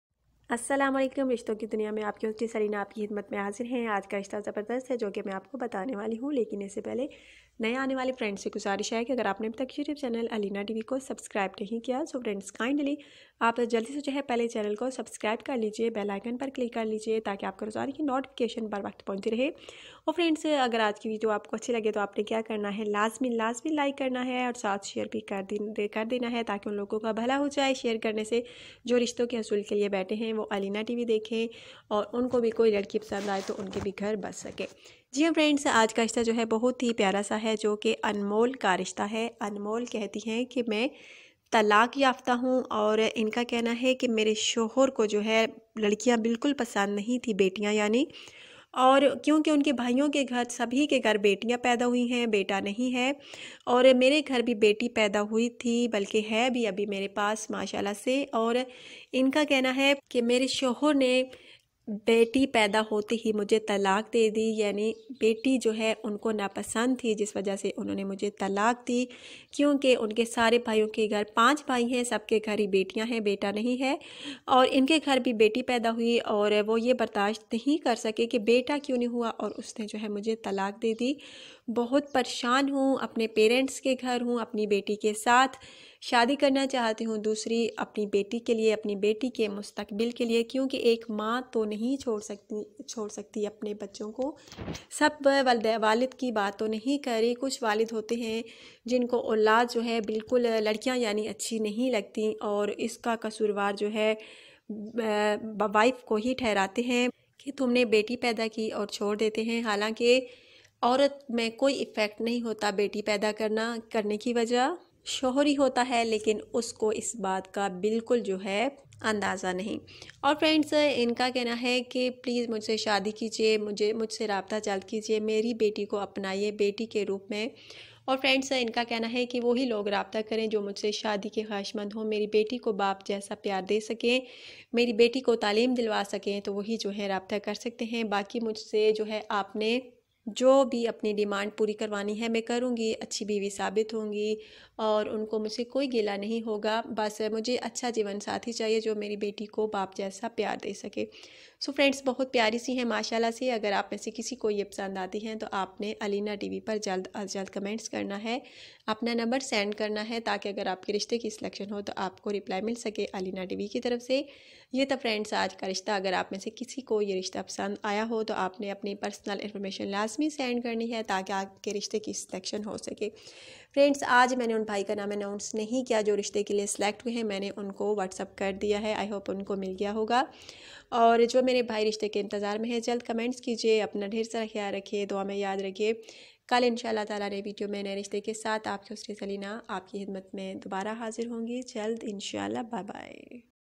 My Name Is Alina I Have A One Daughter Check Details In Urdu And Hindi.
Zaroorat Rishta#
Zaroorat Rishta In All World#
Zaroorat Rishta In All pakistan#
Zaroorat Rishta In 2020#